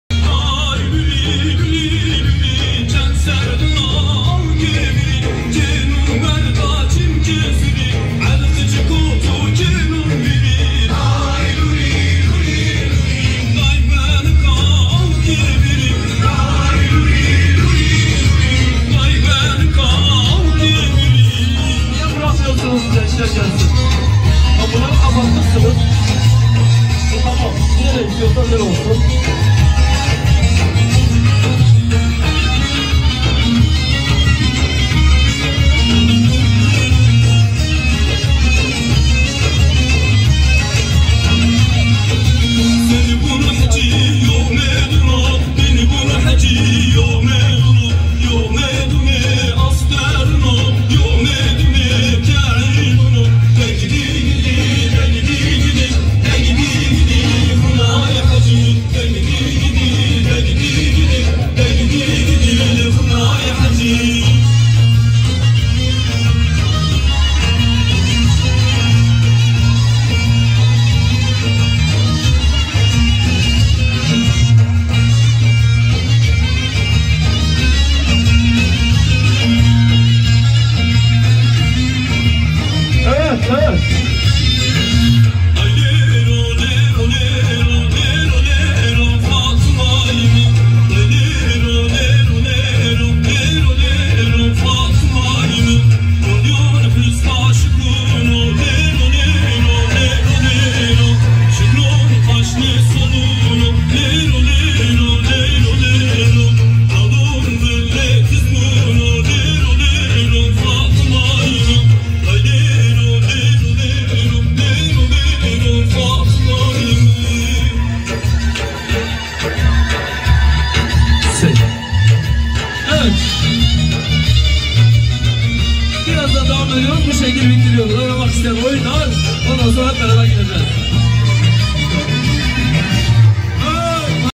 BİNGÖL DE 19 MAYIS KUTLAMASINDA TEKVANDOCULARIN HALAYI
Bingöl de 19 MAYIS kutlamasında çocukların zaman müziği ile halay çekmeleri